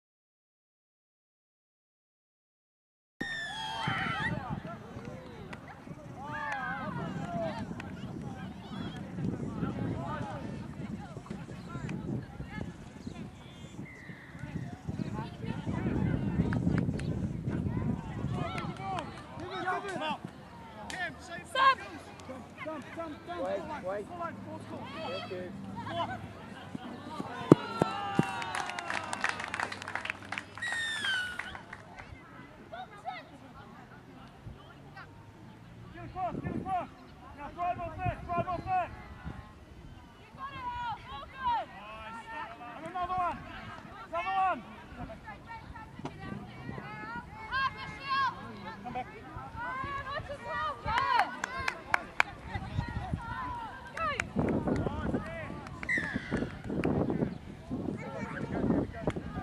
Mich Touch Evolution
Quelques scènes avec l'Open Women's team d'Auckland... application des conseils de Waylon : courir à plein vitesse dans les "arrows", sucer la ligne (avec un essai sympatyque en gros plan)... et puis un essai raté et une passe en avant.